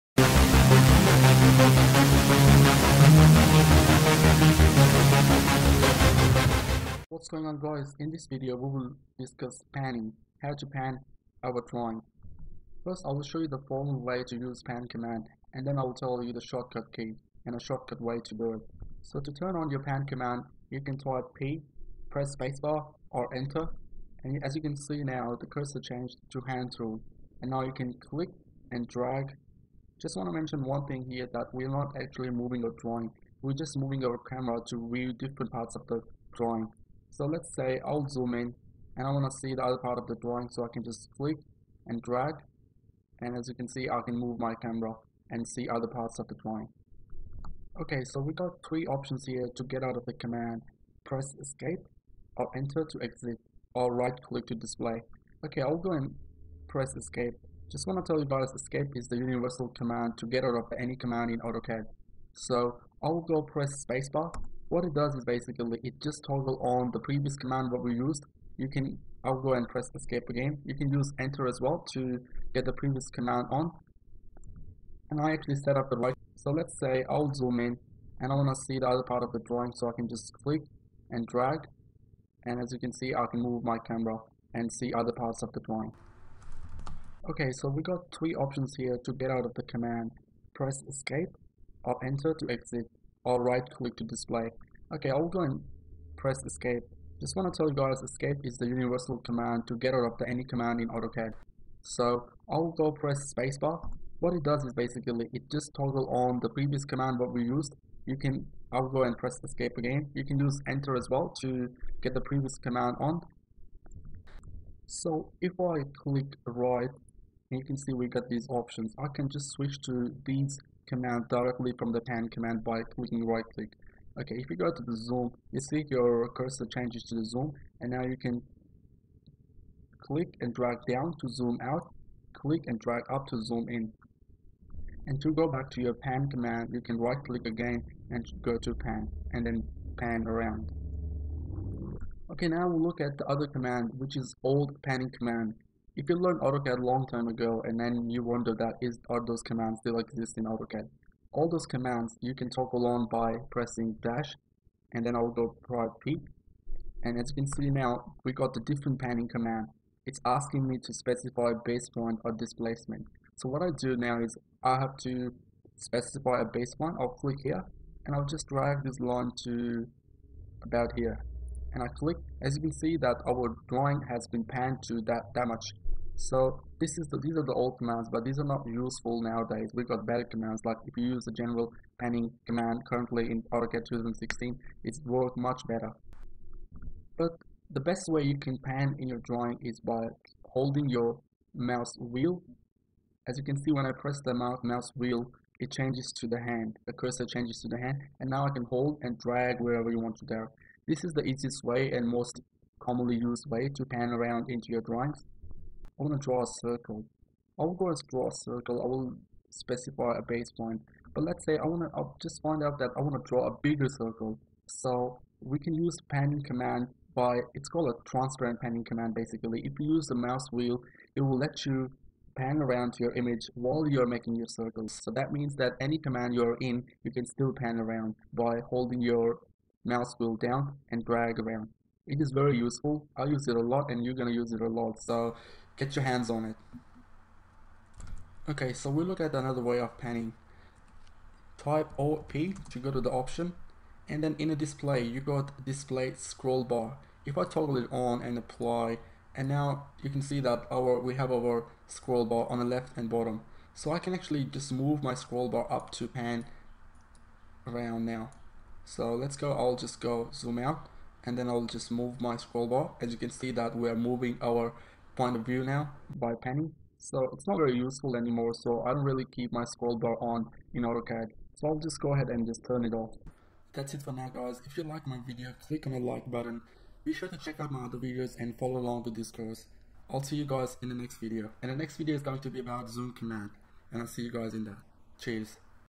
Using Commands and panning in Autocad 2016 training Tutorial 3
Learn Commands Panning In Autocad 2016. in this video i will explain how to pan in Autocad 2016.

Autocad Logo is Been Used for just Teaching Purposes